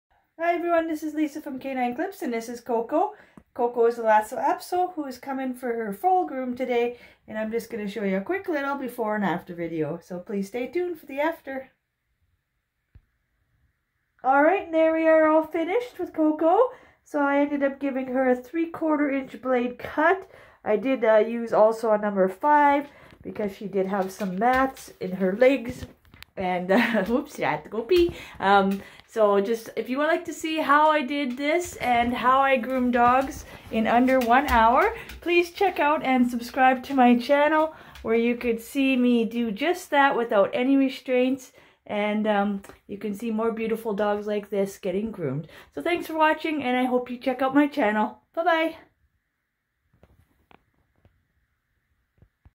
Lhasa Apso dog breed before & after grooming transformation, #3/4 blade, dog grooming
This video shows a a quick before and after video of a Lhasa Apso dog breed that I groomed with #3/4 blade on the body and used scissors on the face.

Each video is unedited footage of a day to day home based dog grooming business. Being a dog groomer for 16+ years, I have seen a lot of dogs and learned a variety of techniques of how to groom any dog WITHOUT restraints!

Lisa
K9 Clipz